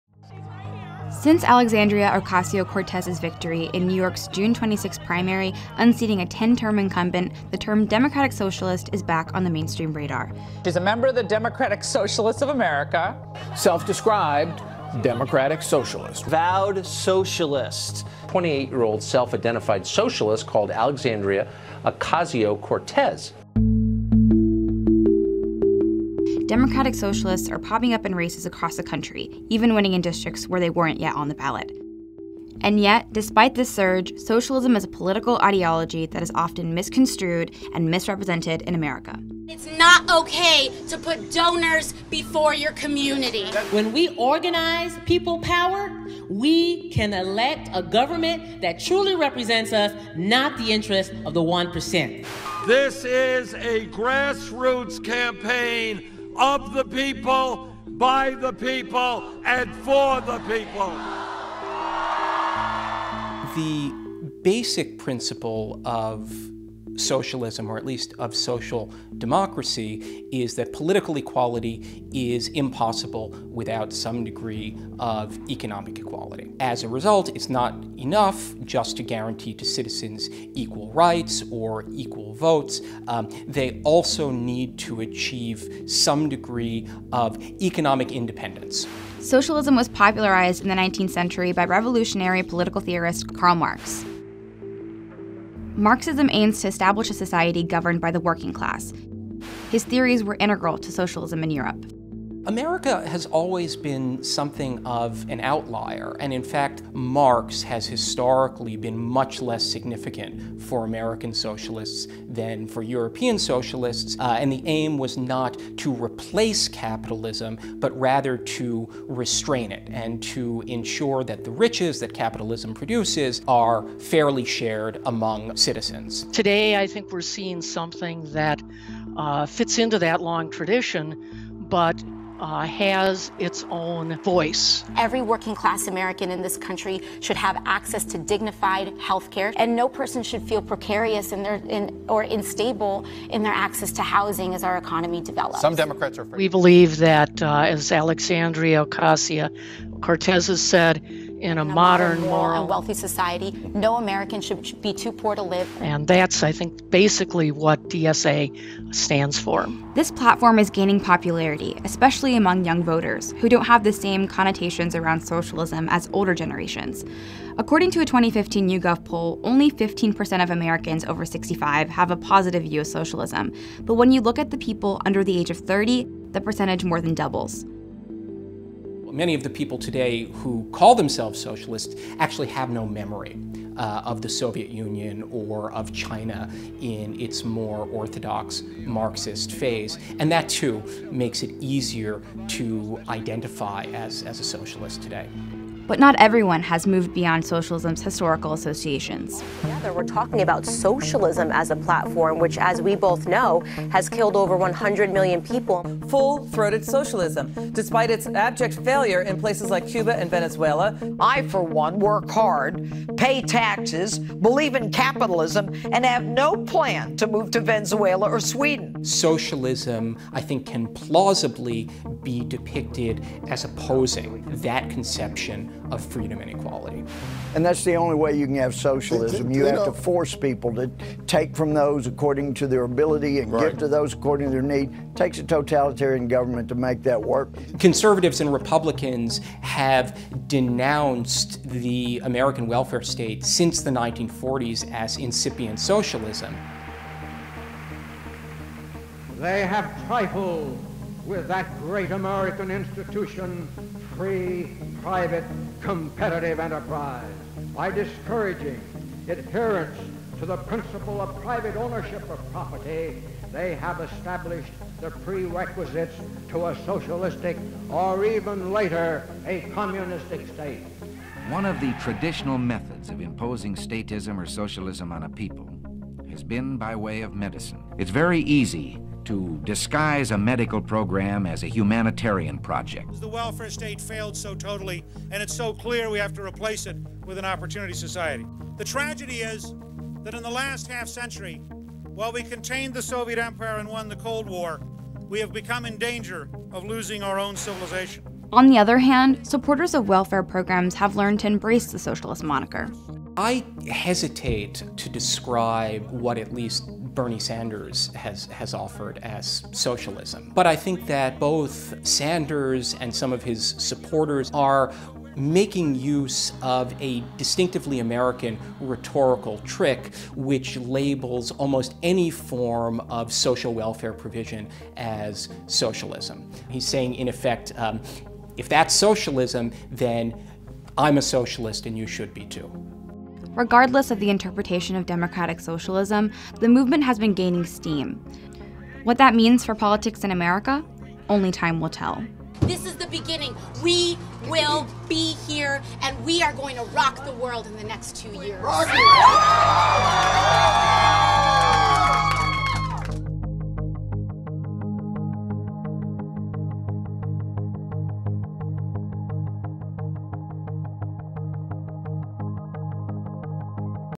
Democratic Socialism, explained
More U.S. candidates are openly calling themselves socialists. From Bernie Sanders to Alexandria Ocasio-Cortez, far-left candidates are getting recognized as serious contenders in races across the country. But socialism is often misunderstood in U.S. politics. So what does it actually mean to be a democratic socialist in America?